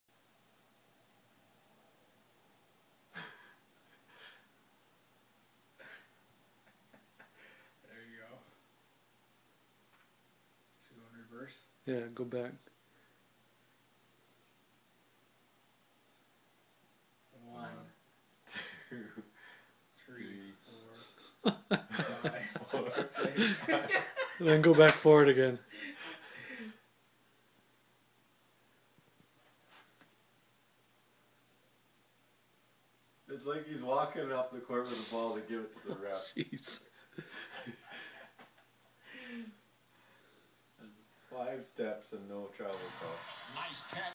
Stiemsma game 7 heat Celtic 6 step travel no call
Ok we all know the nba lets a little traveling go but this was ridiculous.